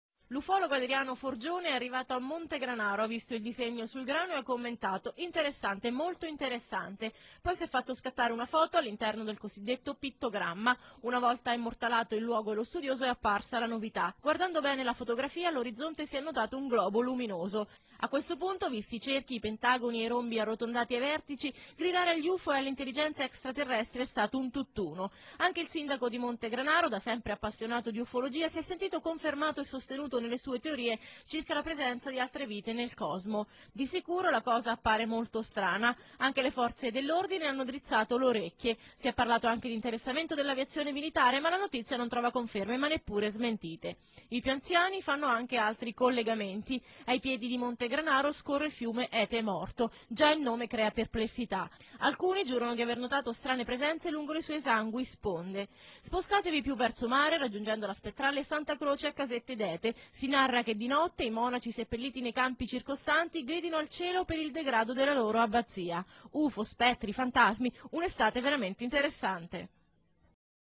Video notizia sui cerchi nel grano di Montegranaro (AP) 2005 [2]
Video notizia sui cerchi nel grano tratto da un articolo del 29 giugno 2005 di